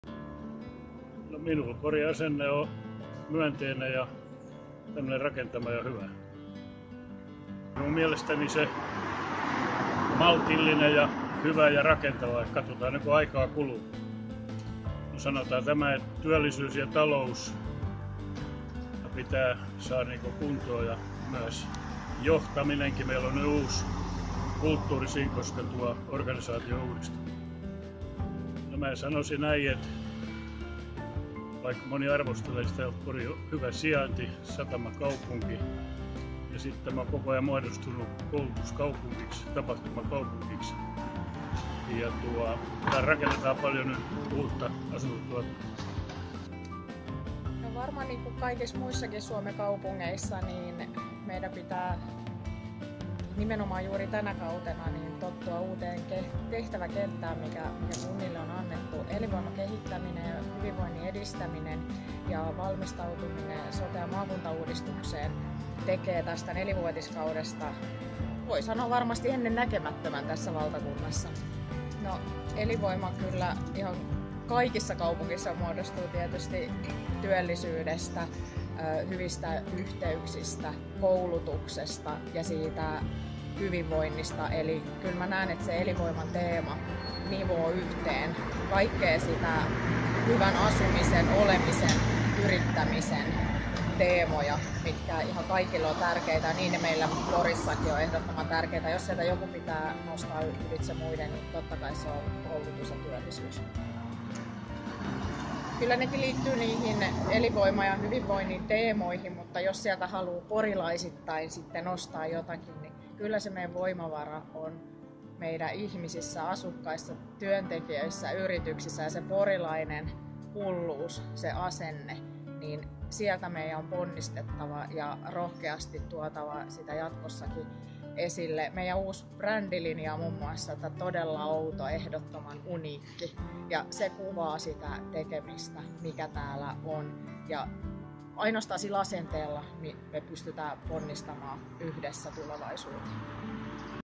Asenne, koulutus ja työllisyys Porin vahvuuksia
Kuntaliiton Kuntasäätiön uusien valtuustojen koulutuskierros Porissa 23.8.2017. Porin kaupunginhallituksen jäsen Esa J. Wahlman sekä kaupunginhallituksen puheenjohtaja Diana Bergroth-Lampinen kertovat, mitä on porilainen asenne.